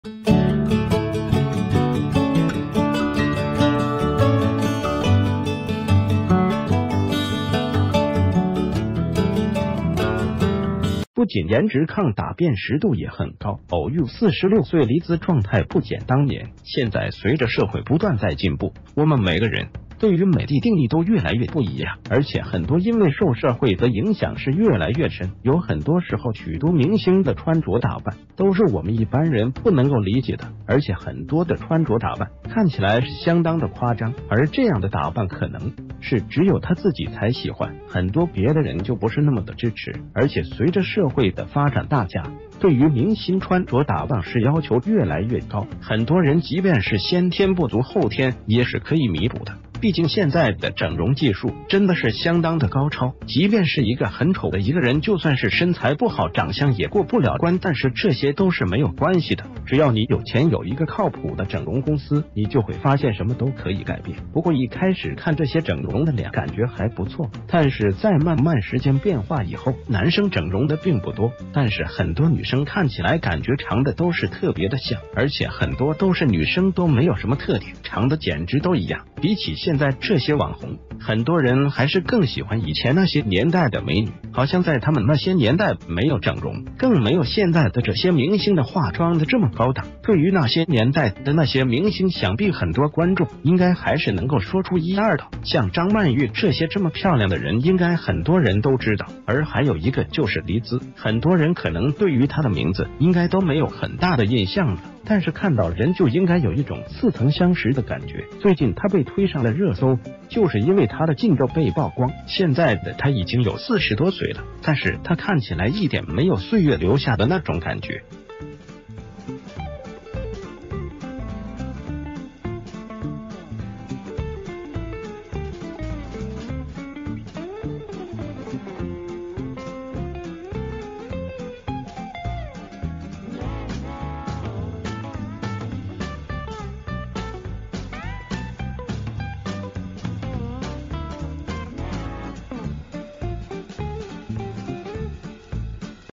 不僅顏值抗打辨識度也很高，偶遇46歲黎姿狀態不減當年
現在隨著社會不斷在進步我們每個人對于美的定義都越來越不一樣，而且很多因為受社會的影響是越來越深，有很多時候許多明星的穿著打扮都是我們一般人不能夠理解的，而且很多的穿著打扮看起來是相當的夸張，而這樣的打扮可能是只有她自己才喜歡，很多別的人就不是那么的支持。
    而且隨著社會的發展大家對于明星穿著打扮是要求越來越高，很多人即便是先天不足后天也是可以彌補的，畢竟現在的整容技術真的是相當的高超。
    即便是一個很丑的一個人就算是身材不好長相也過不了關但是這些都是沒有關系的，只要你有錢有一個靠譜的整容公司你就會發現什么都可以改變。
    不過一開始看這些整容的臉感覺還不錯，但是在慢慢時間變化以后，男生整容的并不多，但是很多女生看起來感覺長的都是特別的像，而且很多都是女生都沒有什么特點，長的簡直都一樣。
    比起現在這些網紅，很多人還是更喜歡以前那些年代的美女，好像在她們那些年代沒有整容更沒有現在的這些明星的化妝的這么高檔。
    對于那些年代的那些明星想必很多觀眾應該還是能夠說出一二的，像張曼玉這些這么漂亮的人應該很多人都知道。
    而還有一個就是黎姿，很多人可能對于她的名字應該都沒有很大的印象了，但是看到人就應該有一種似曾相識的感覺。
    最近她被推上了熱搜就是因為她的近照被曝光，現在的她已經有40多歲了，但是她看起來一點沒有歲月留下的那種感覺，看起來是相當的年輕，真的是那個年代過來的顏值真的是抗打。